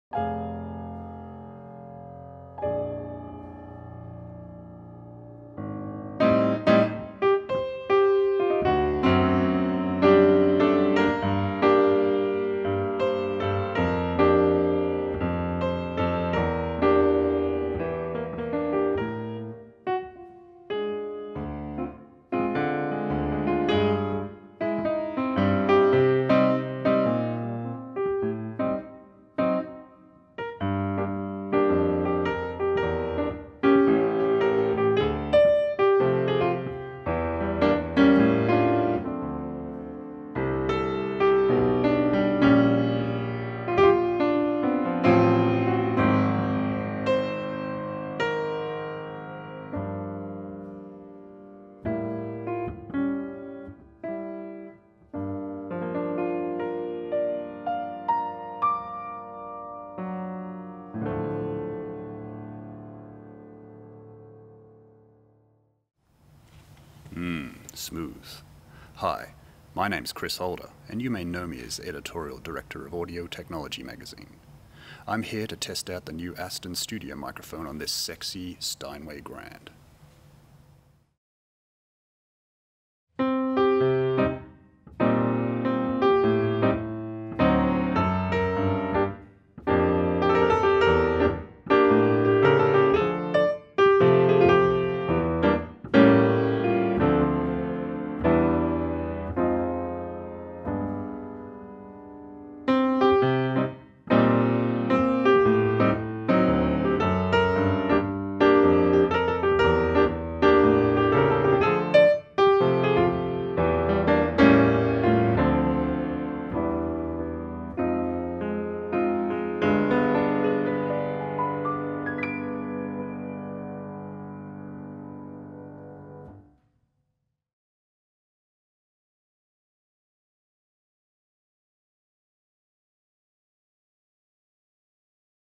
Aston Origin vs Rode NT1 on a Steinway
Hear the new British Aston Microphones Origin compared with AudioTechnology's standard for affordable condenser mics — the Rode NT1.

Listent to and download the raw comparison files for each mic

To stay updated with news, reviews and features on the pro audio world, head over to: